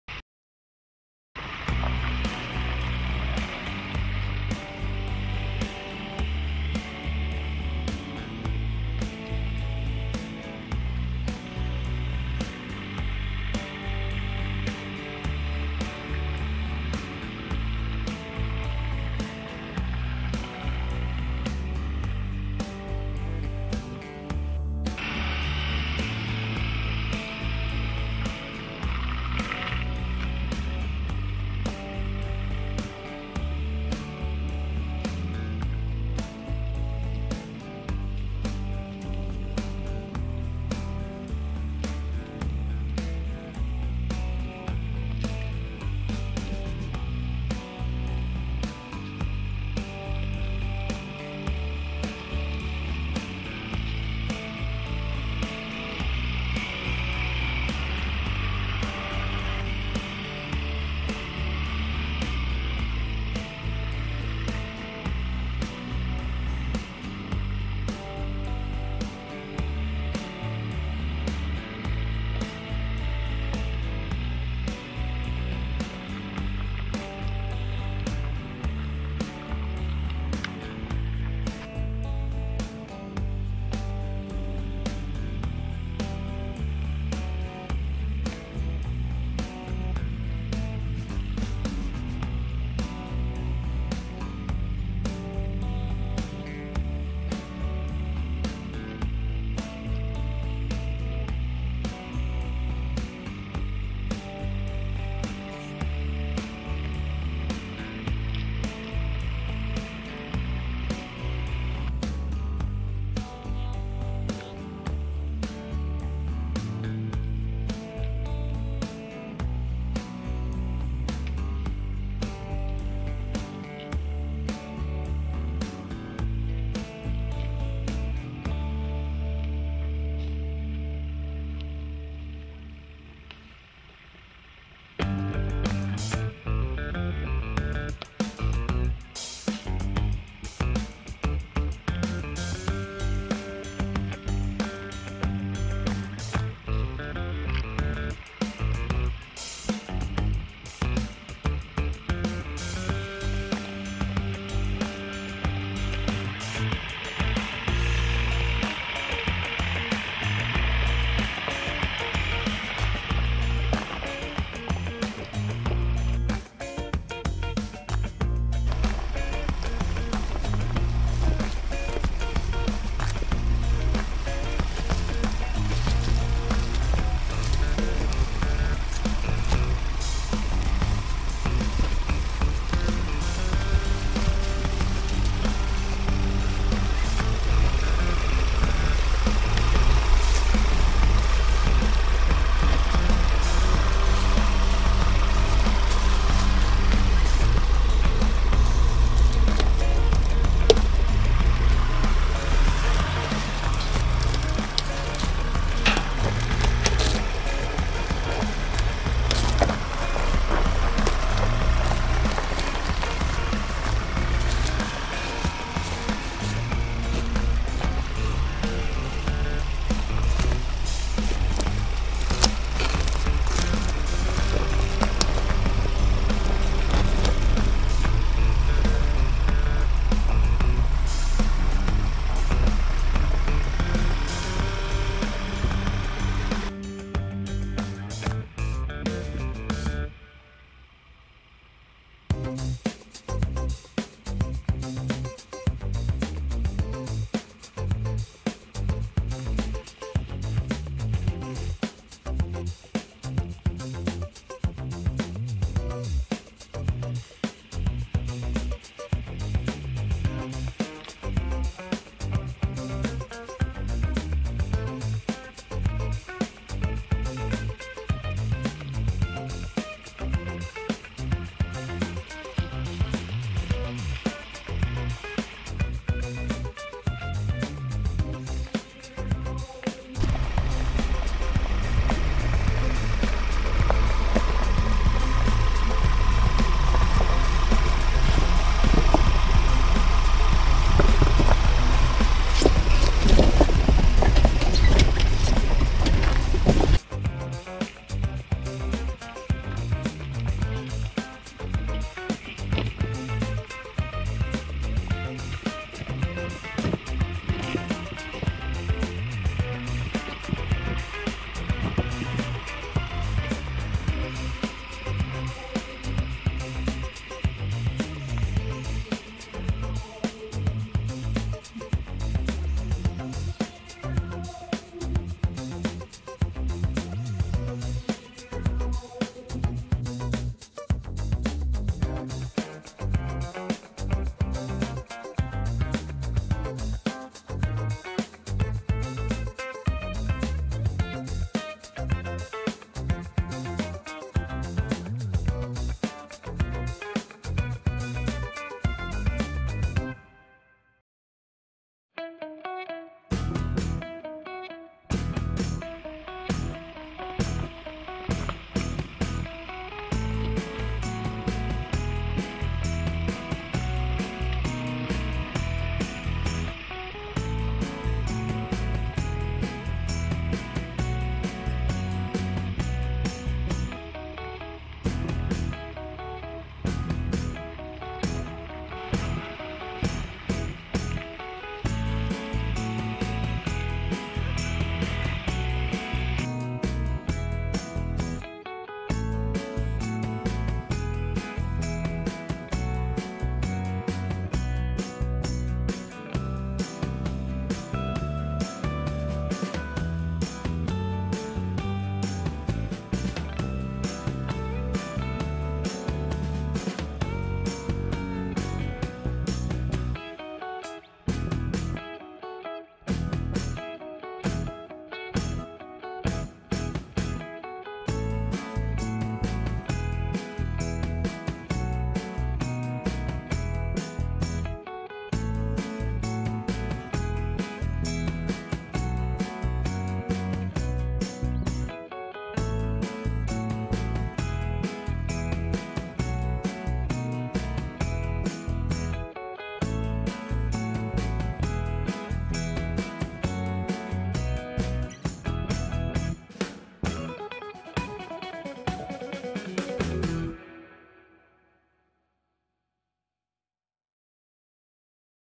Onlinemotor Ford Ranger Raptor offroad im VSZ Olpe
Vergleicht man des Raptor mit dem normalen Ford Ranger oder jedem anderen modernen Pick Up verkörpert der Ranger Raptor die Ausstrahlung eines bestens austrainiertester 10 Kämpfer vor dem Wettkampf.